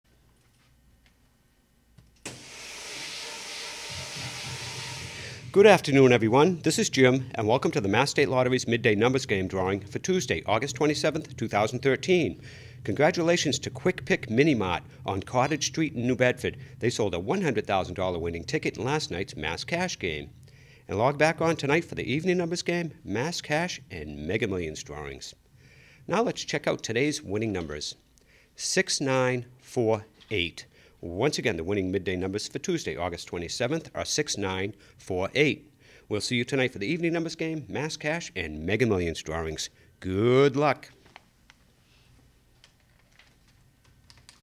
Midday Numbers Game Drawing: Tuesday, August 27, 2013
The official winning numbers in the Mass. State Lottery's Midday Numbers Game drawing for Tuesday, August 27, 2013 are: 6-9-4-8.